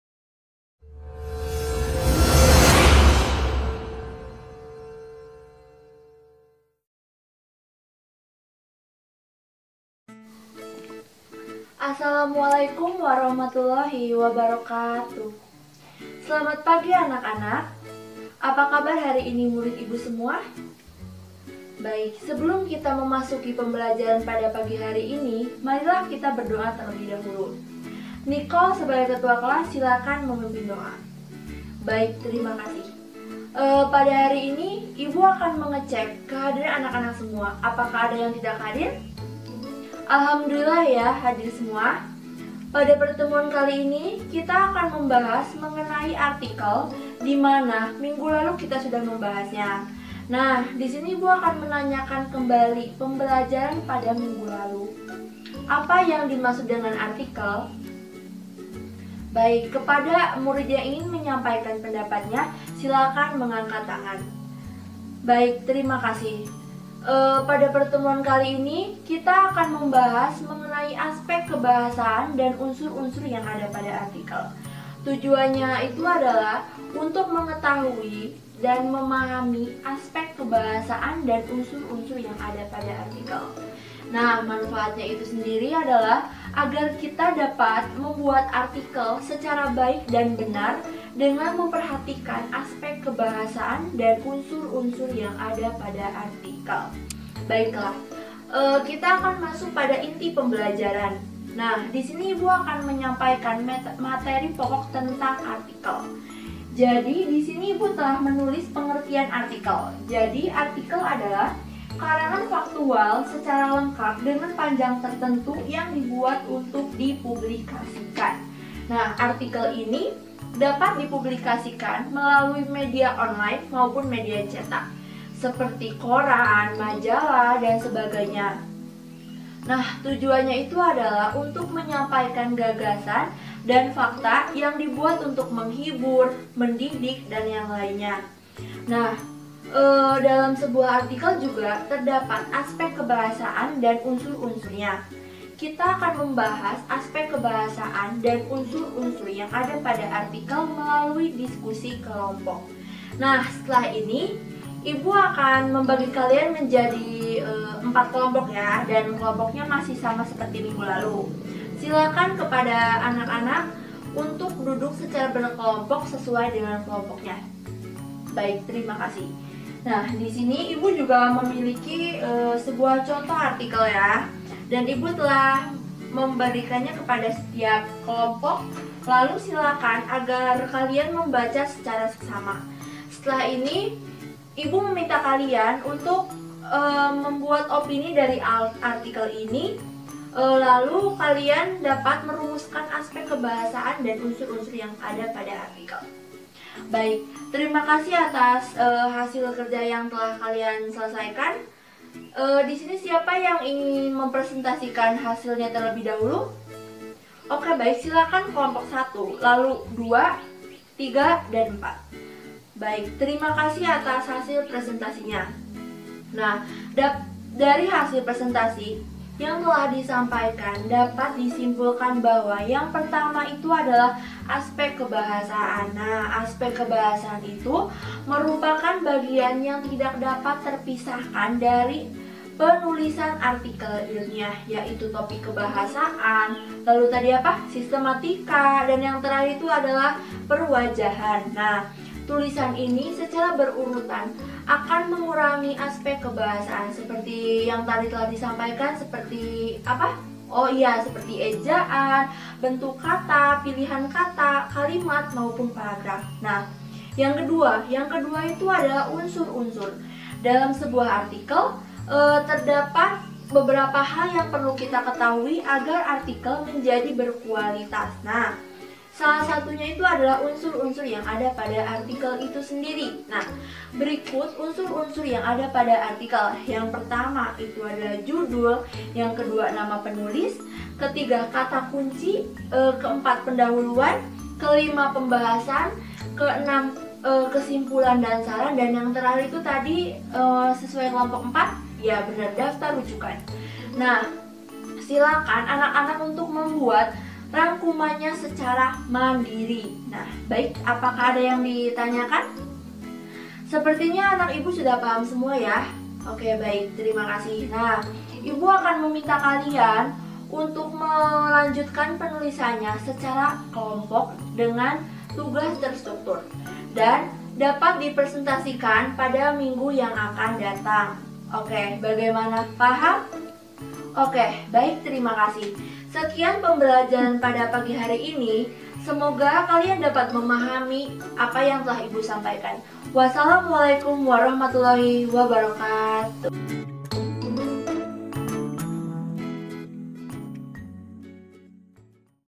1022_Lina Nur Aeni_2B
Praktik mengajar untuk memenuhi UAS mengenai "Arti Tentang Artikel"